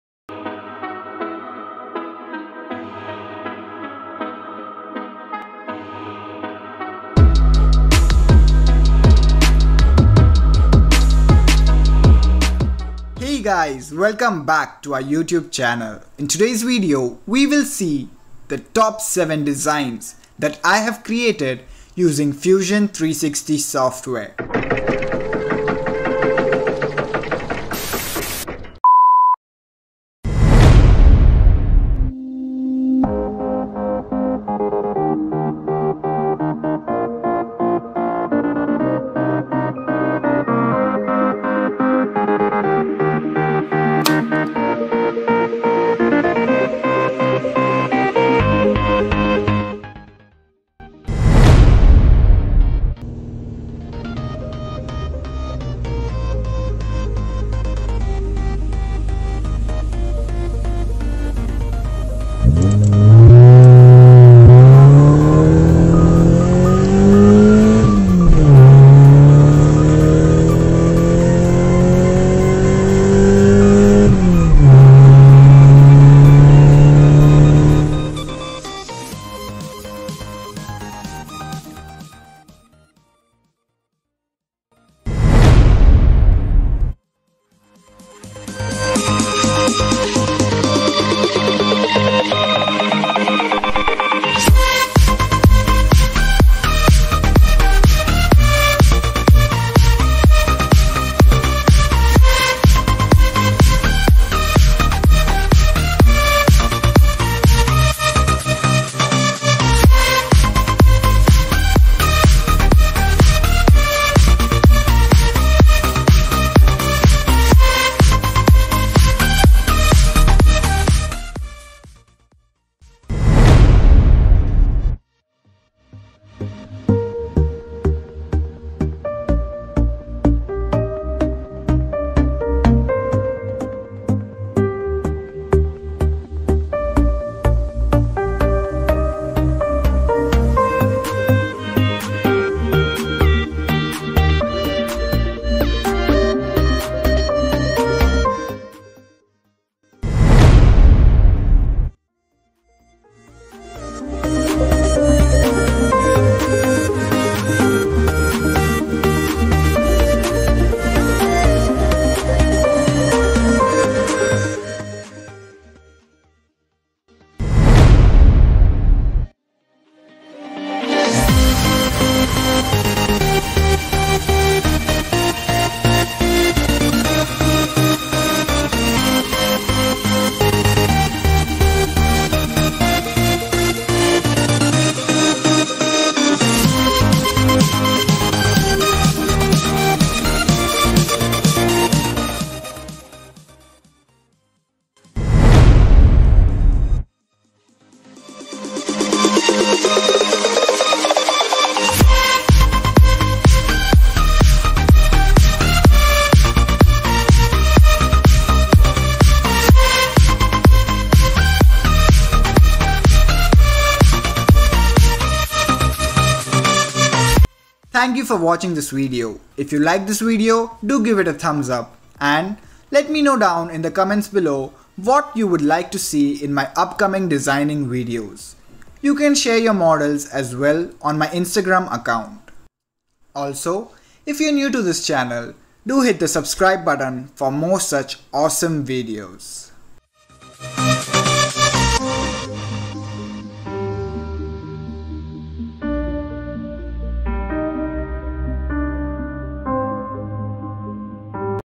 My TOP 7 3D models in fusion 360
This video is a collection of my top 7 models which are created using Fusion360 software.

Sit back and enjoy this compilation video with some soothing background beats.